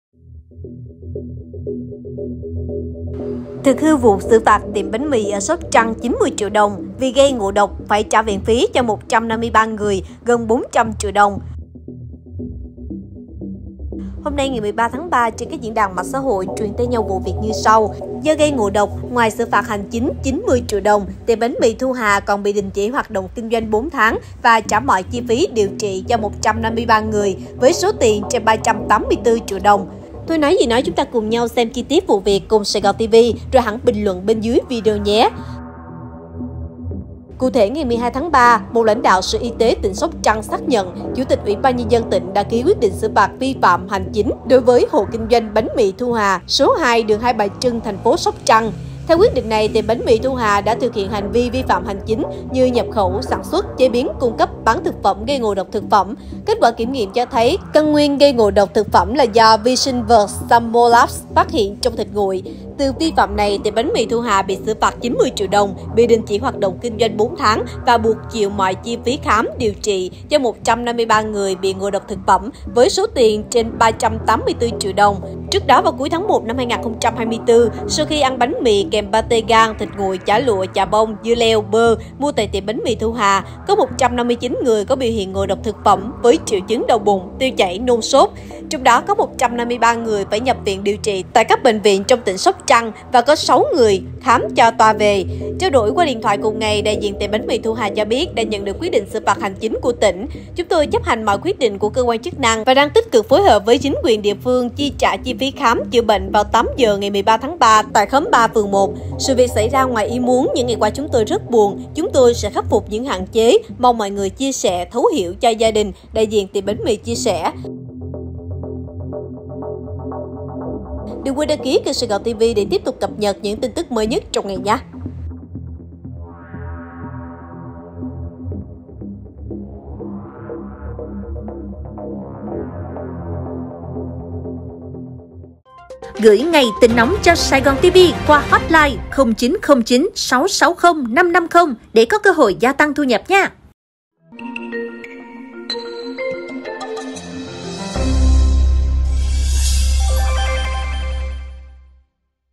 Xử phạt tiệm bánh mì GÂY NGỘ ĐỘC ở Sóc Trăng 90 TRIỆU… trả viện phí cho 153 người GẦN 400 TRIỆU
Trong đó có 153 người phải nhập viện điều trị tại các bệnh viện trong tỉnh Sóc Trăng và 6 người khám cho toa về. Trao đổi qua điện thoại cùng ngày, đại diện tiệm bánh mì Thu Hà cho biết đã nhận được quyết định xử phạt hành chính của tinh.
| Theo Tuổi Trẻ | Video mang tính minh họa